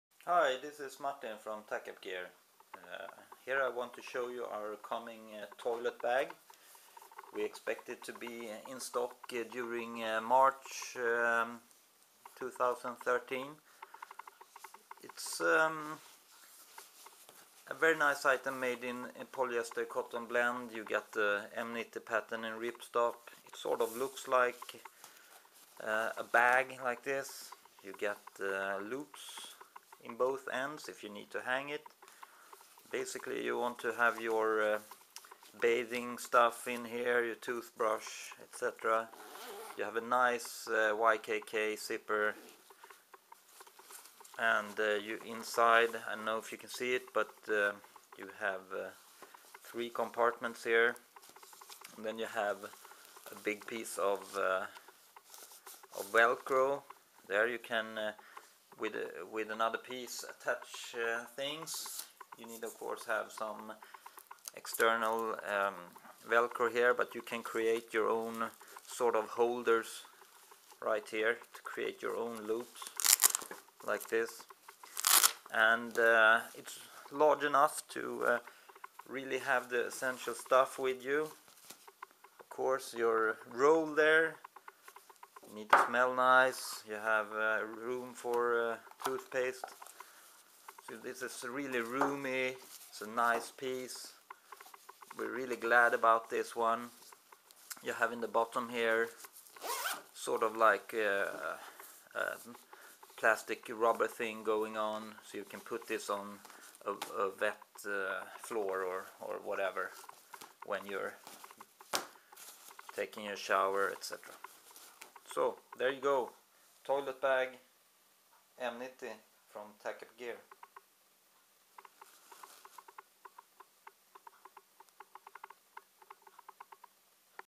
Tac-Up Gear Necessär / Toilette Bag M90
Necessär / Toilet bag in unique look in rip stop M90 pattern from Tac-Up Gear